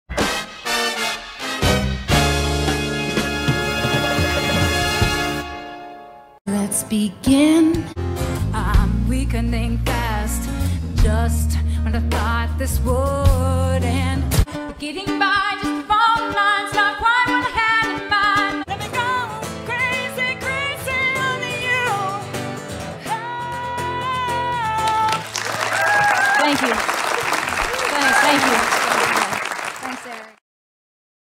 University Heights Summer Concert Series presents Diana Chittester
Diana Chittester makes her Summer Concert Series debut at Walter Stinson Community Park on Thursday, July 8th.